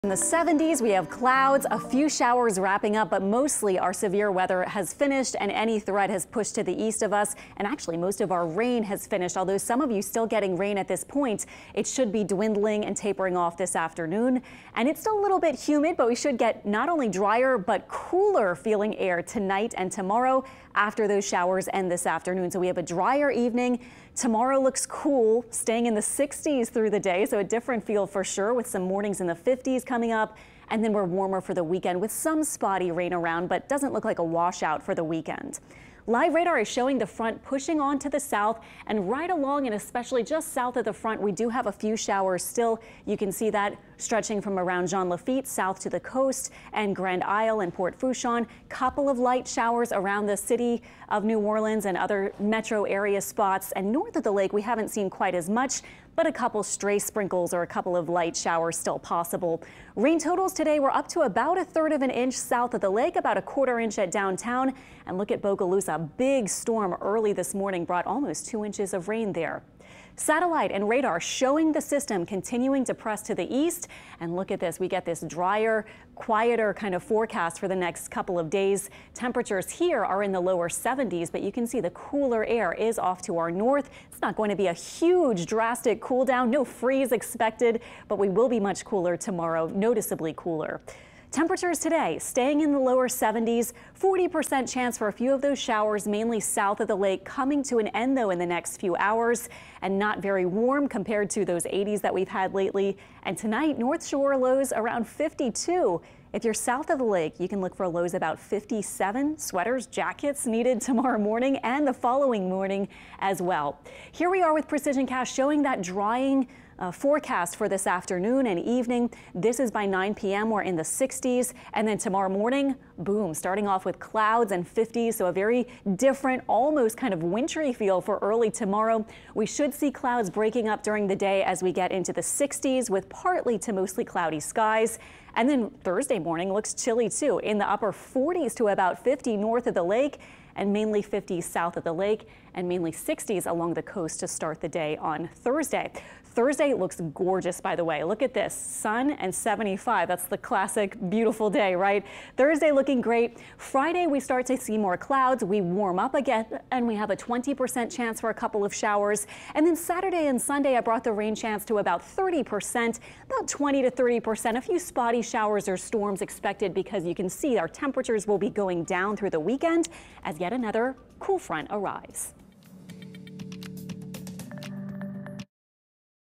Showers wrap up, turning cool Tuesday night
Meteorologist Alexandra Cranford has the forecast at noon on Tuesday, March 28, 2023.